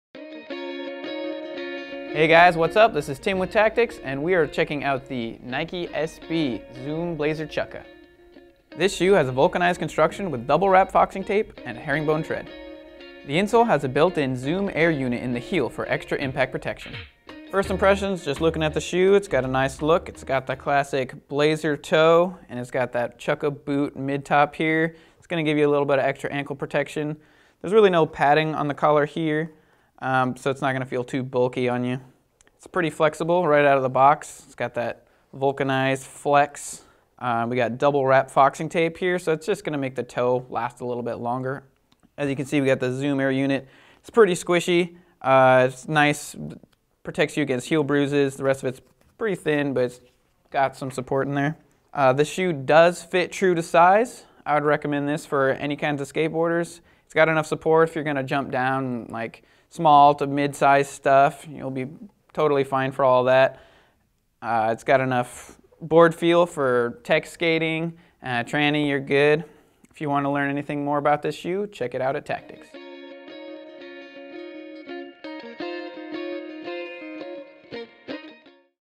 Nike SB Zoom Blazer Chukka Skate Shoes Review | Tactics
These Blazer Chukkas are skate-ready right out of the box. Designed similar to the classic Blazer, these Chukkas have a flexible vulcanized construction with herringbone tread for lasting grip and boardfeel. A Zoom Air unit in the sockliner gives you responsive support and the height of the Chukkas gives you a little extra ankle protection.